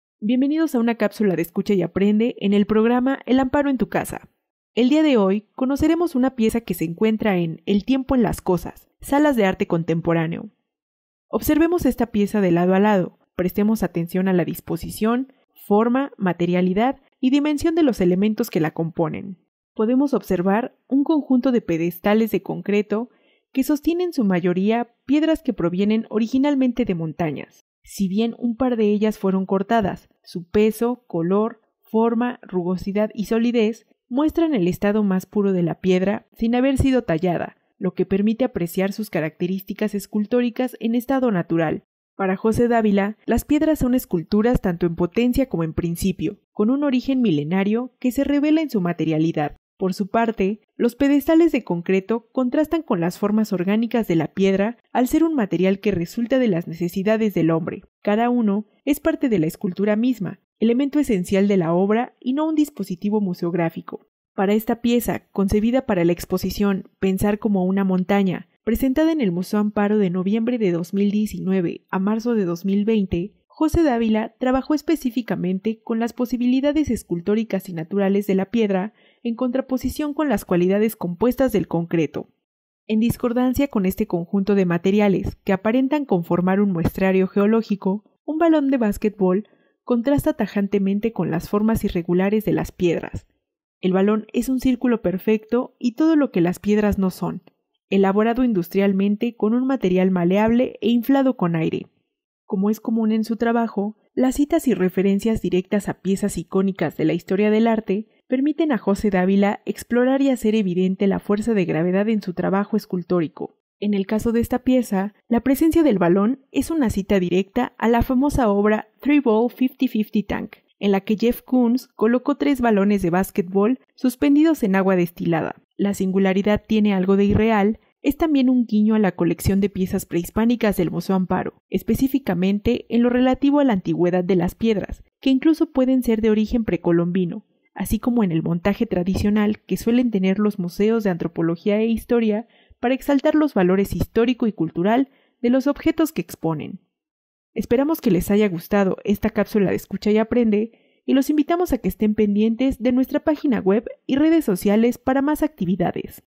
La singularidad tiene algo de irreal
En esta pieza, Jose Dávila hace referencia a la materialidad y organicidad de la piedra en contraposición con los materiales creados por el hombre. Conócela escuchando esta cápsula.